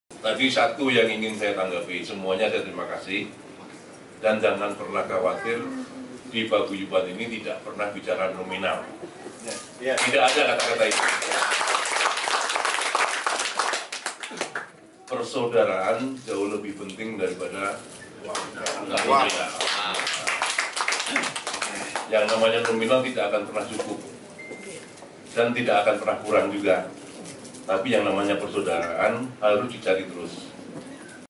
Pesan Ki lurah Punggowo
Pesan Ki lurah Punggowo Bahwa Persaudaraan itu jauh lebih penting daripada Nominal